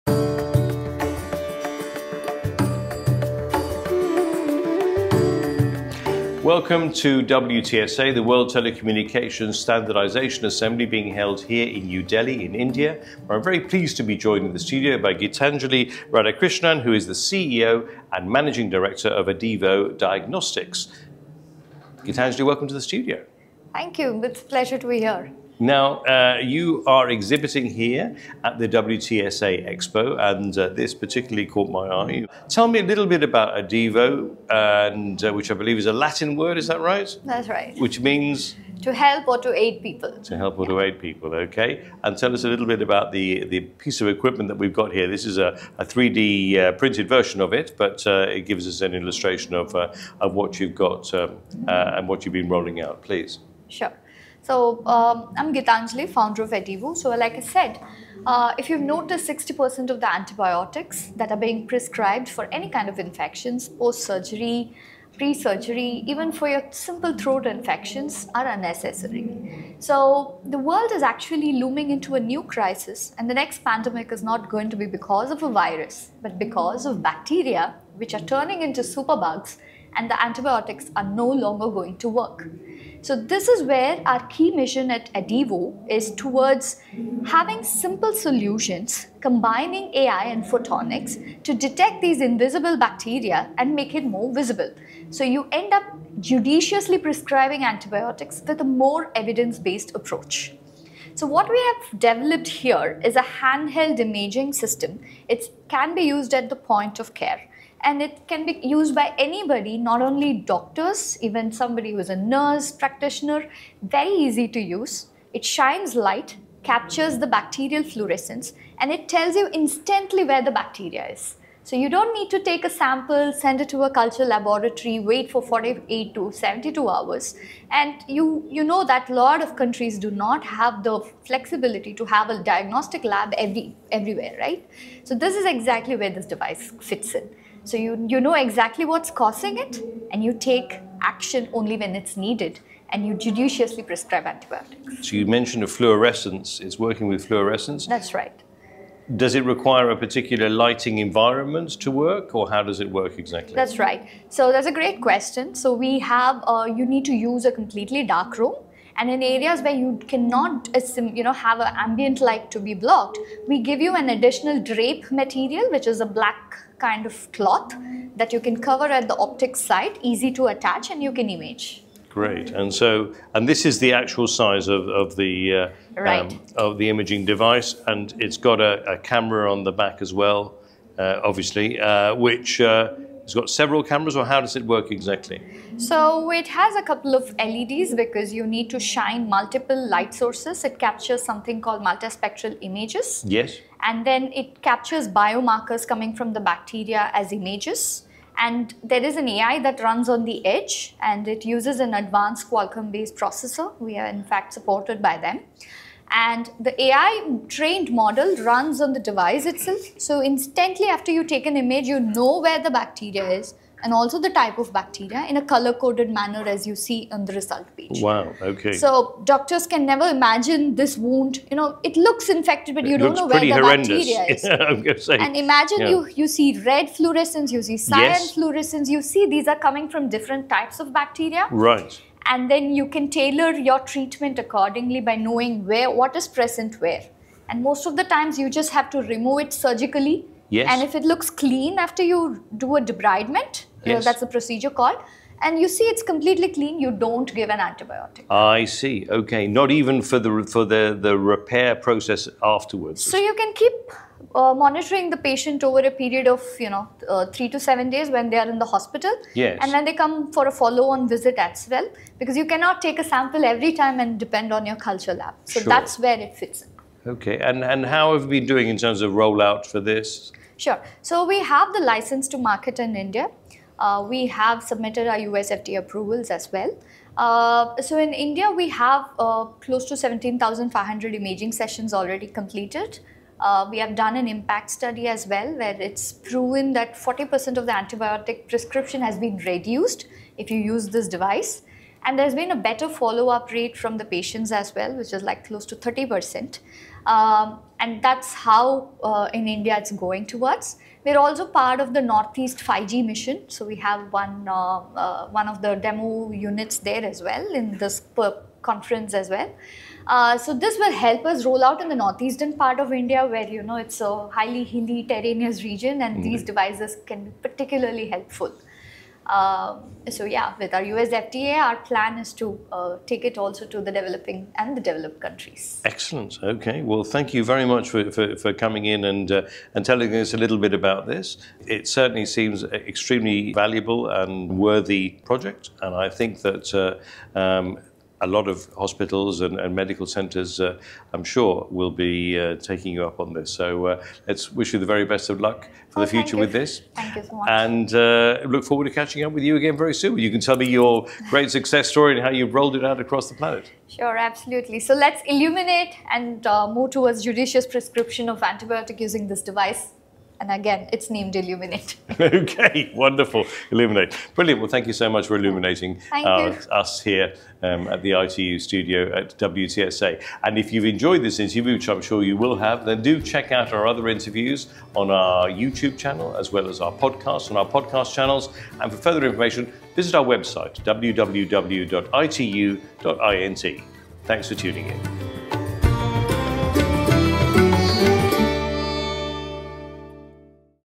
ITU INTERVIEWS @ WTSA-24: Geethanjali Radhakrishnan, CEO & Managing Director, Adiuvo Diganostics
Interview with Geethanjali Radhakrishnan, CEO & Managing Director, Adiuvo Diganostics, at the World Telecommunication Standards Symposium 2024 (WTSA-24), 14 – 24 October 2024, New Delhi, India.